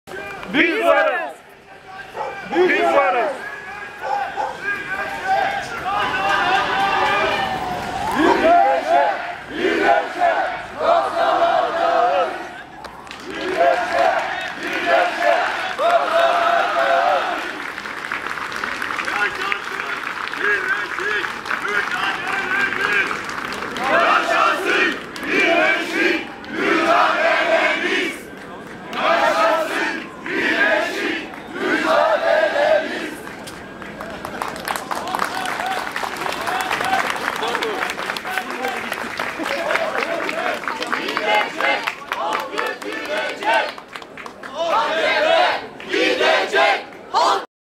Birleşik Haziran Hareketinin İzmir Buluşması
Birleşik Haziran Hareketinin Gelişi, Aralık Ayında Sokakları Isıtıyor